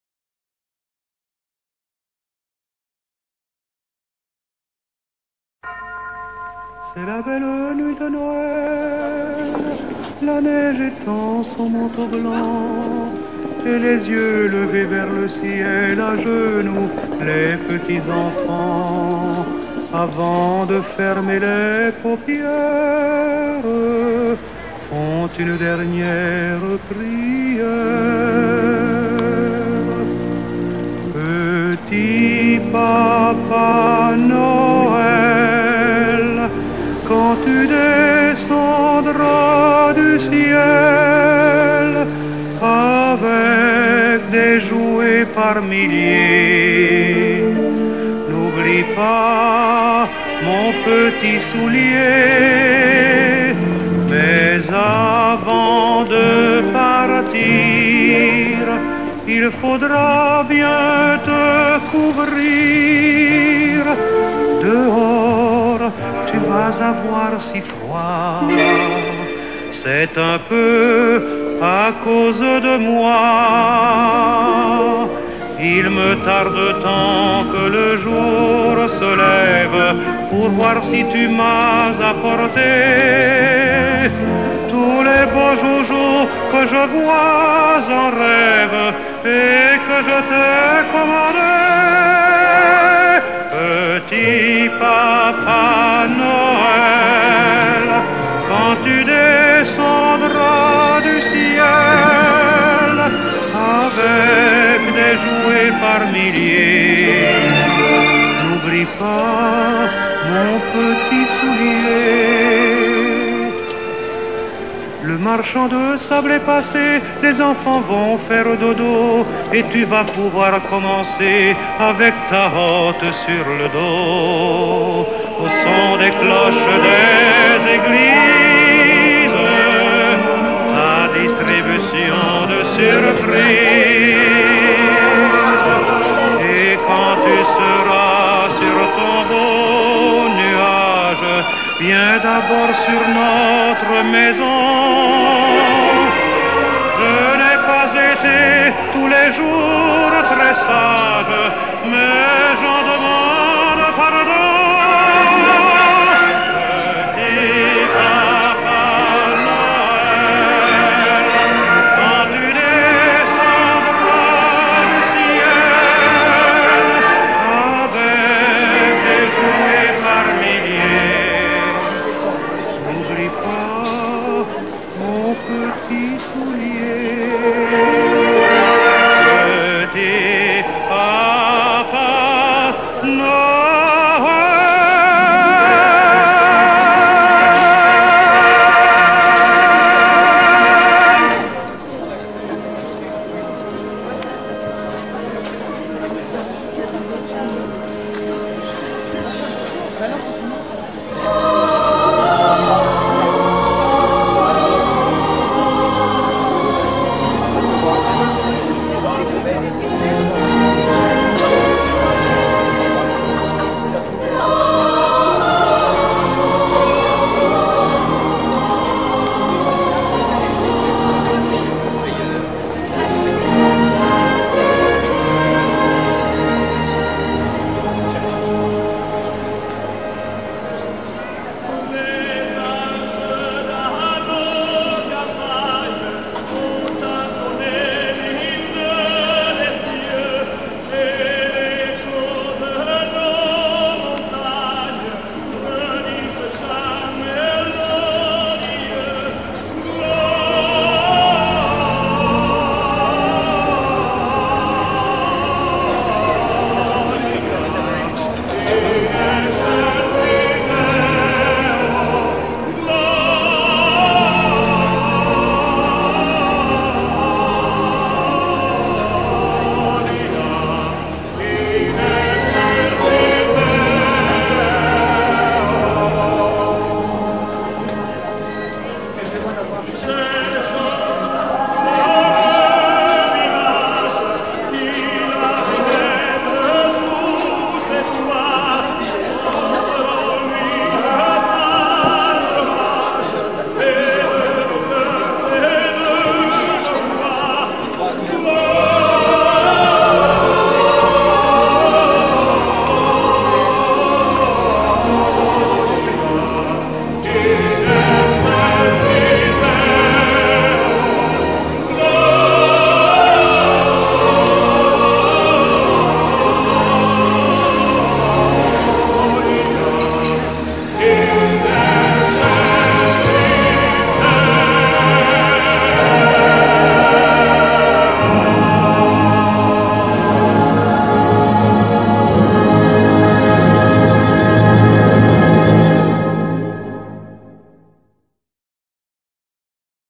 Le marché de noël de Mulhouse
Retrouvez toute la magie de noël à travers ces images des illuminations et du marché de noël de Mulhouse en 2012. Un joli souvenir !